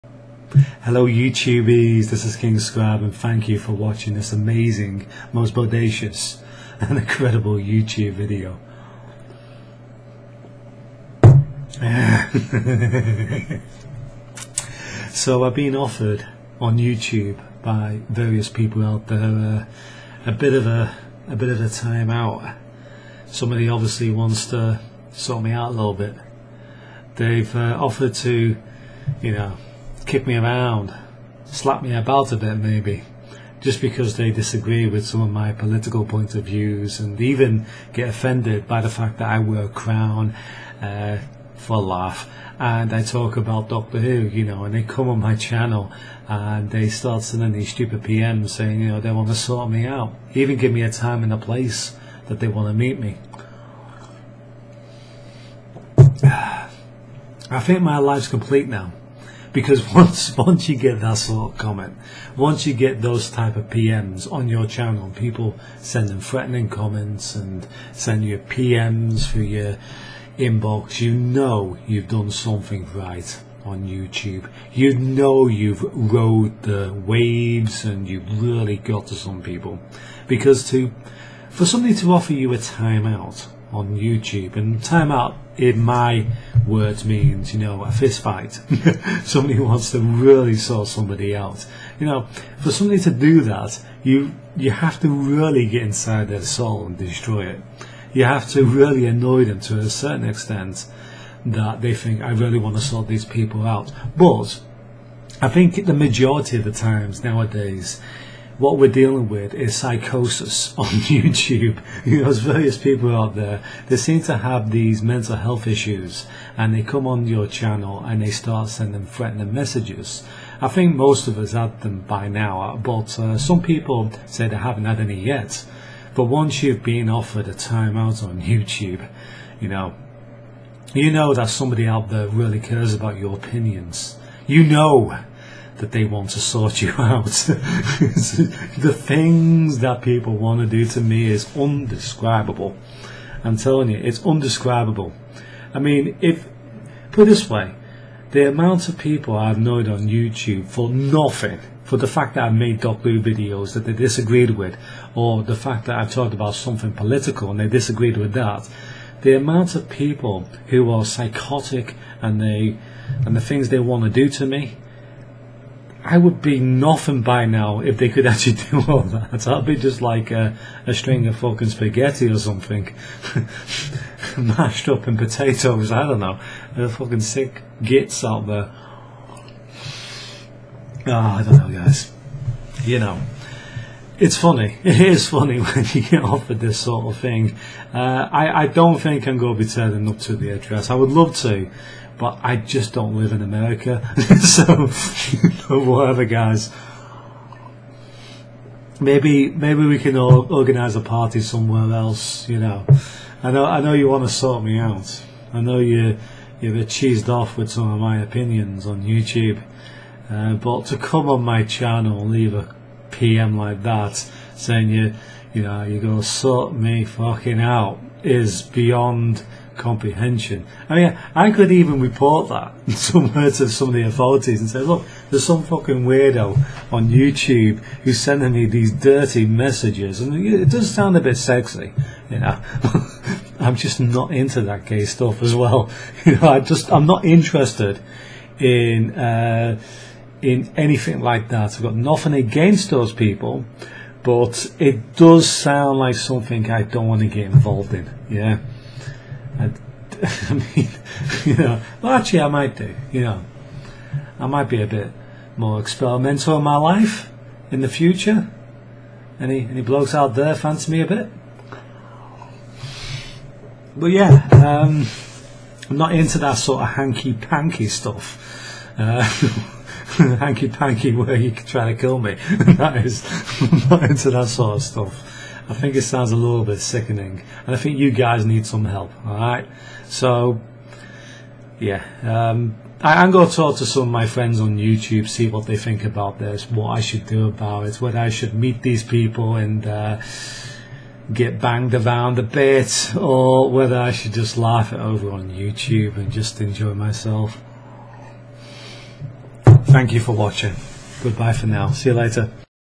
I've Been Threatened Online By 5 Year Old Trolls - Facking Yeah Man!!!
5 year old Trolls threaten to beat the crap out of me, because I'm a Doctor Who fan and support ALS and other charities, because I am such an angel.  etc. I'm so not scared.